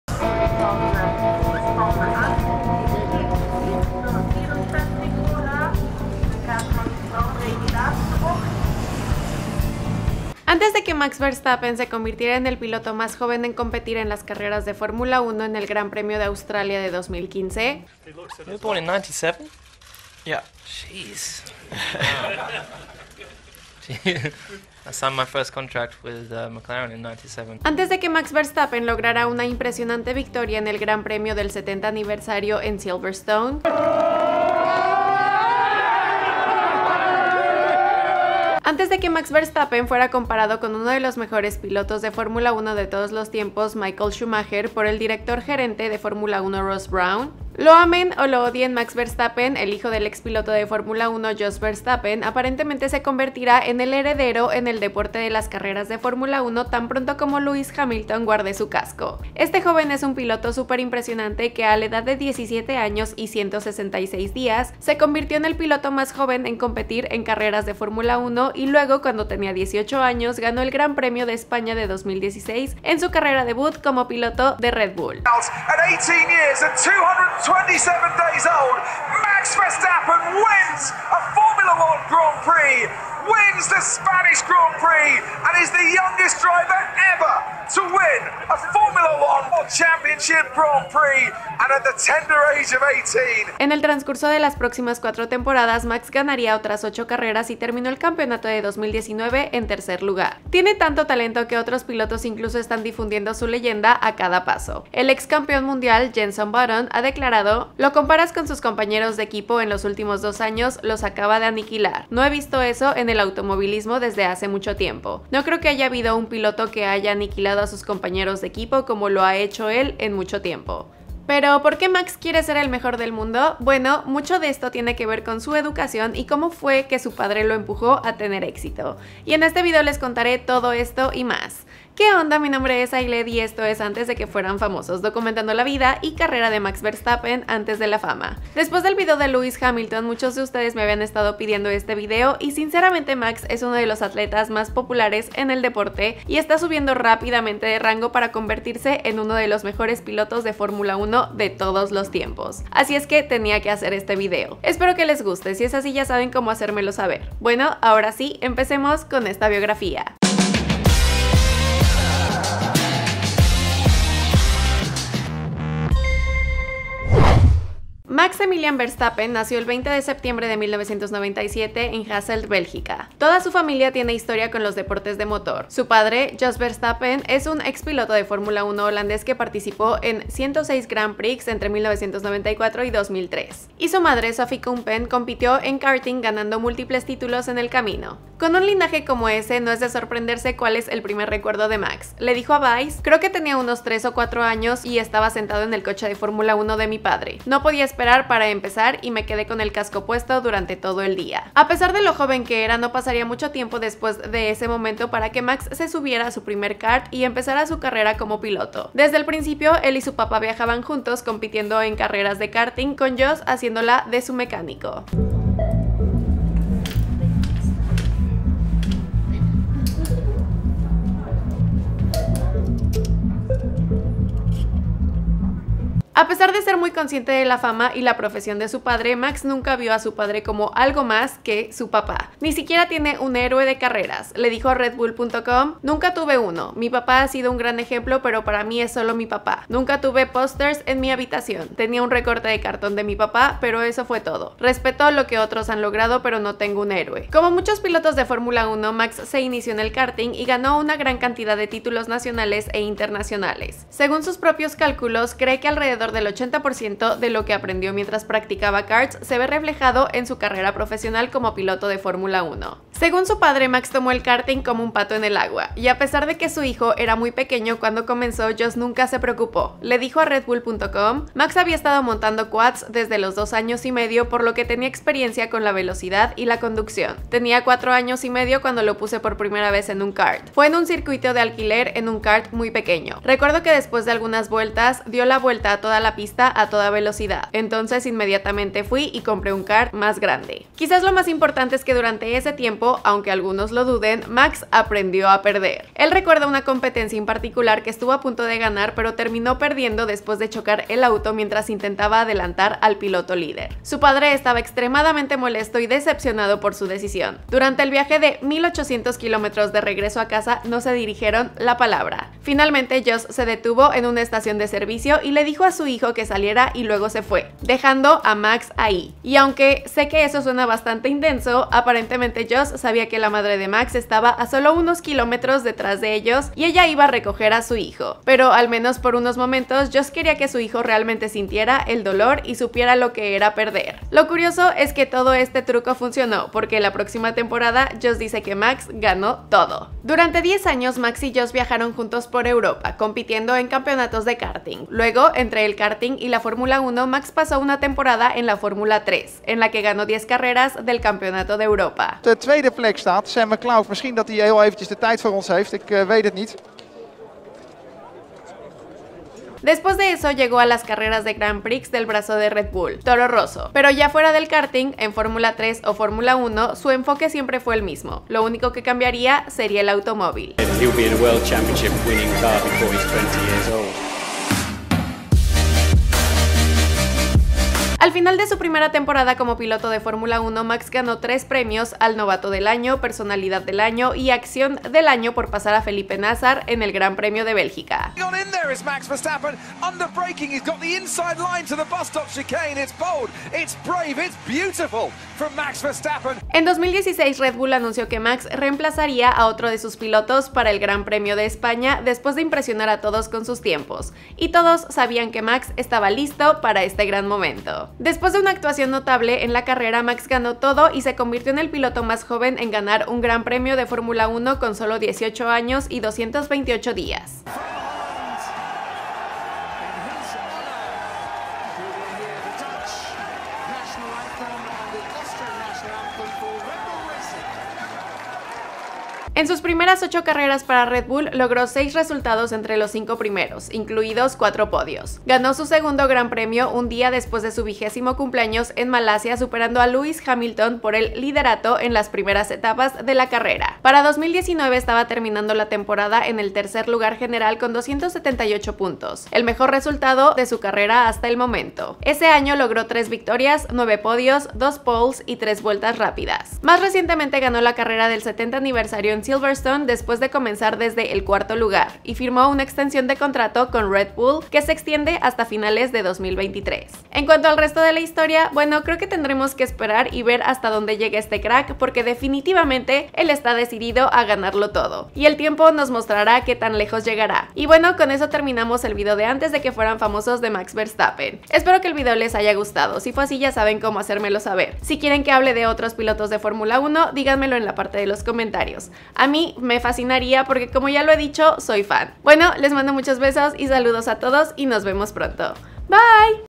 Max Verstappen | Antes De Que Fueran Famosos | F1 Red Bull | Biografía Español
Max Verstappen es un piloto de Fórmula 1 que ha alcanzado el éxito